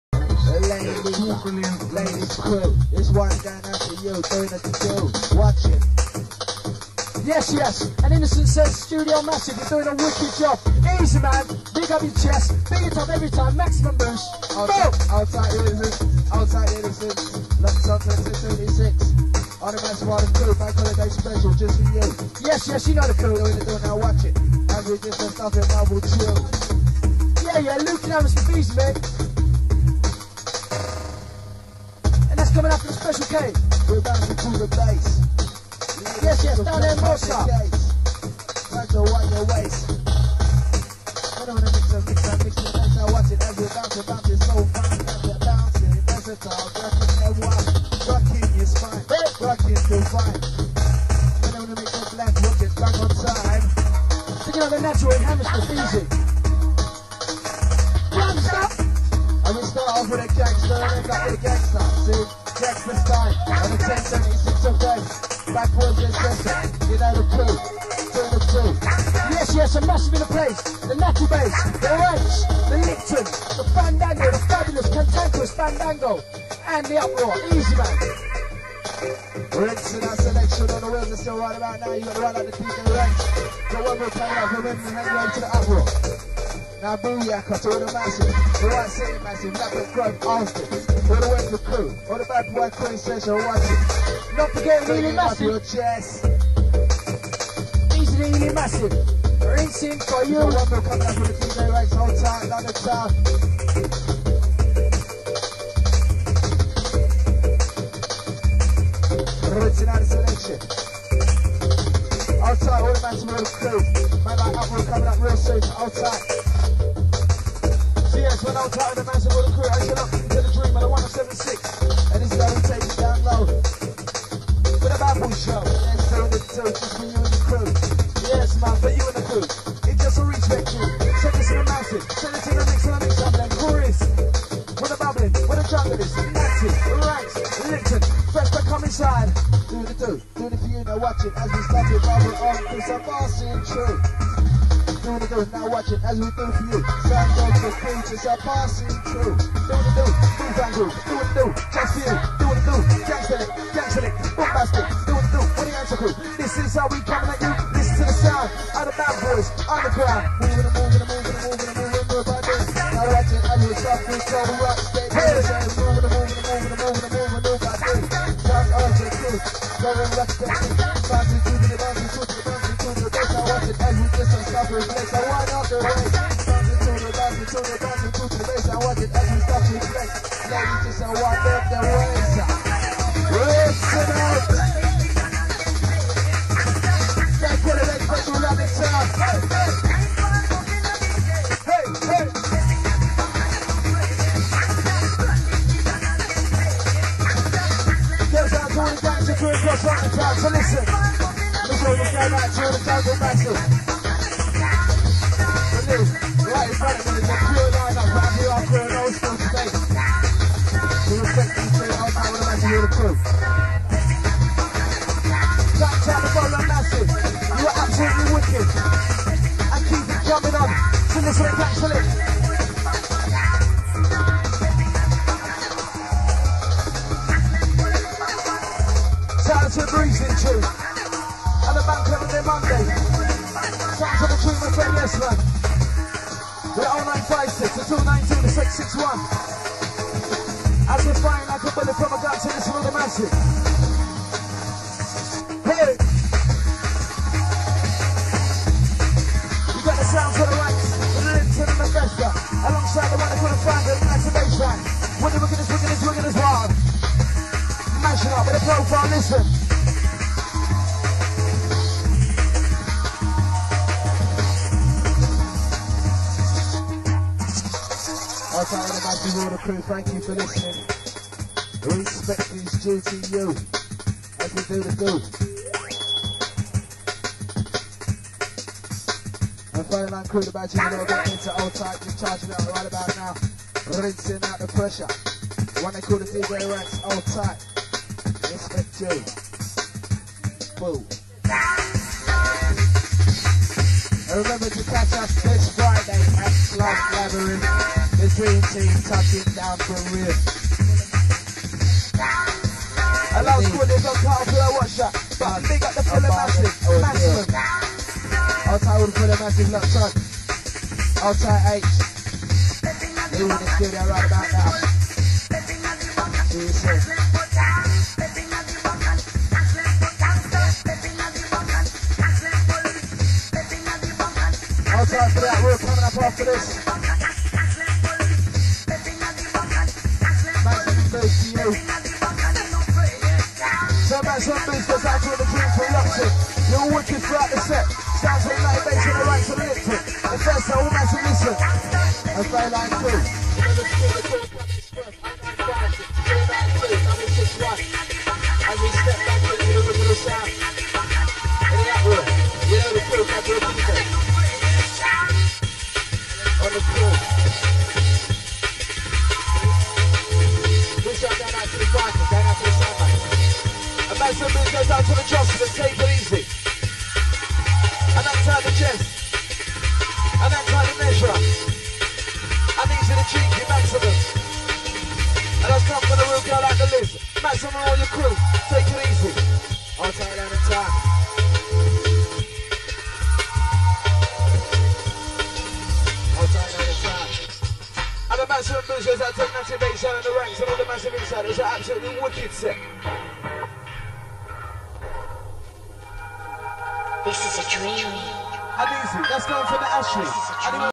Dream FM 107.6 Awesome Jungle part 8
The final part of this jungle set from a Bank Holiday Monday back in 1995 from Dream FM